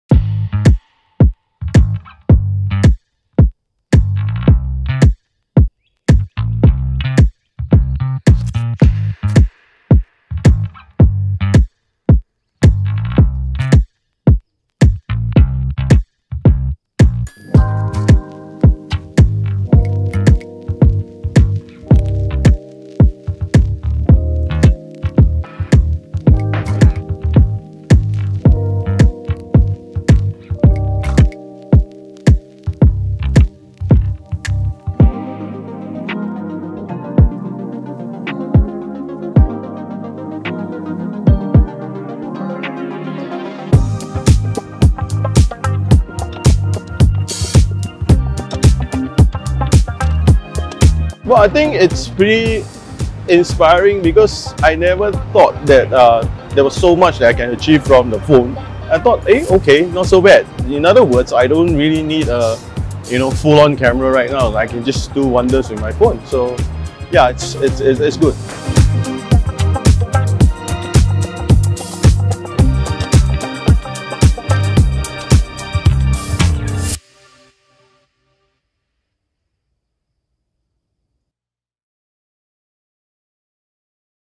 Sleepless in SG Exhibition Highlights​
The night has never looked so bright at the SleeplessinSG exhibition. Check out the highlights to see the master photographer Geoff Ang at work, epic 360 videos and more. It’s amazing how you can MakeNightsEpic with the Nightography feature on the Galaxy S22 Series 5G.